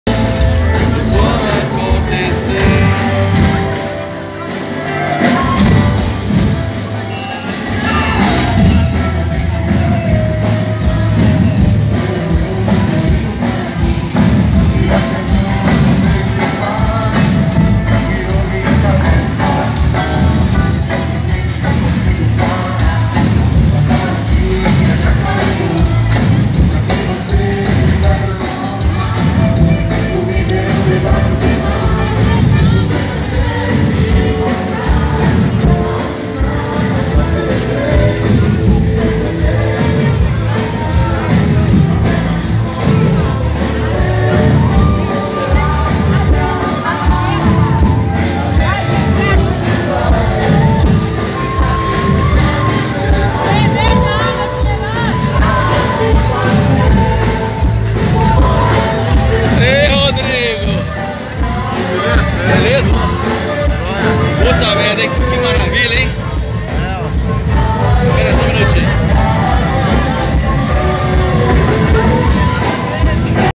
CARNAVAL 2024 - BH - Evento Teste - 14/01/24 - Av. dos Andradas - BLOCO ESQUINA - SANTA TEREZA
BLOCO ESQUINA - SANTA TEREZA - BELO HORIZONTE - MINAS GERAIS